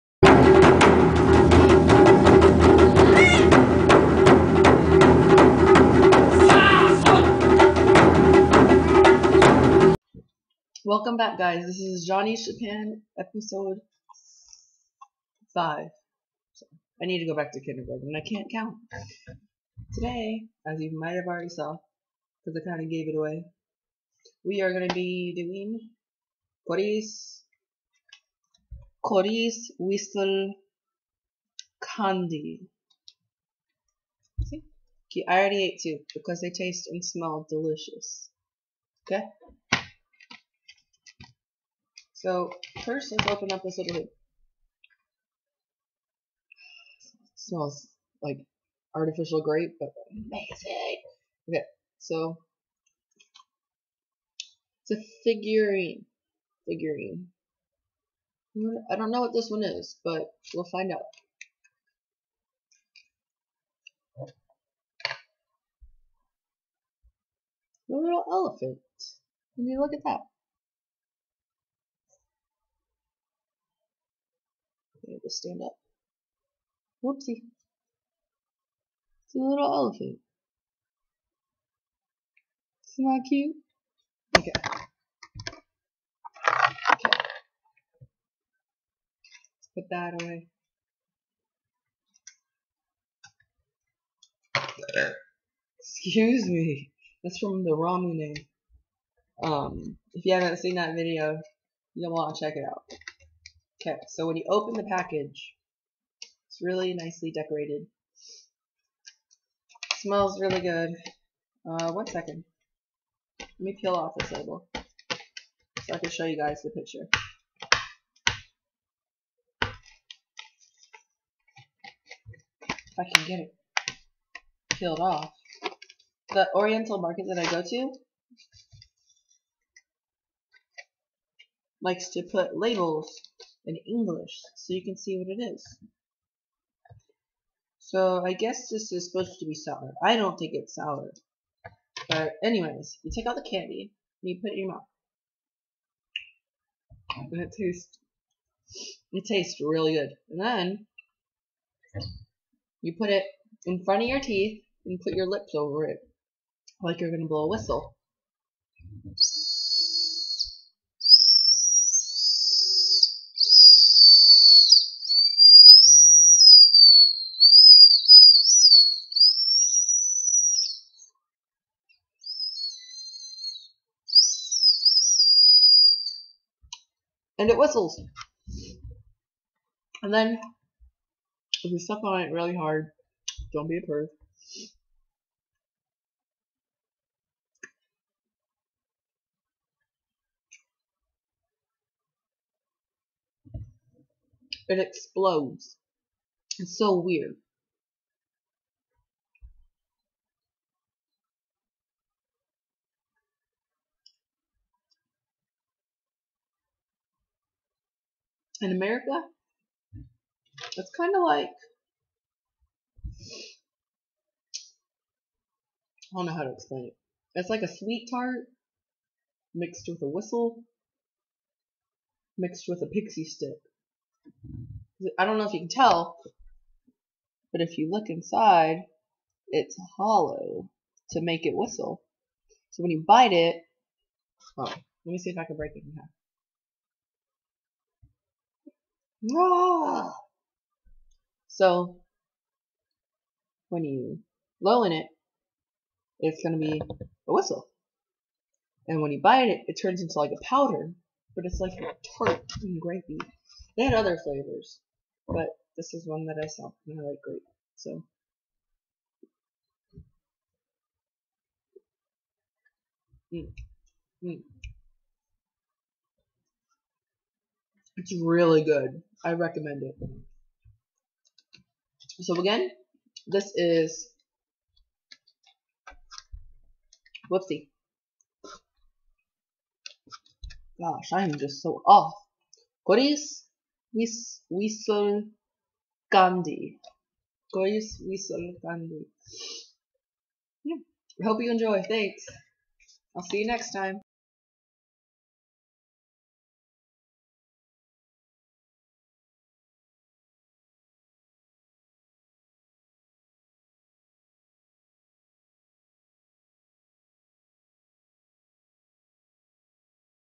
Cordis Whistle Candy - JoneatsJapan episode 5
a brief review on Cordis Whistle Candy, a candy that really whistles... and its japanese!!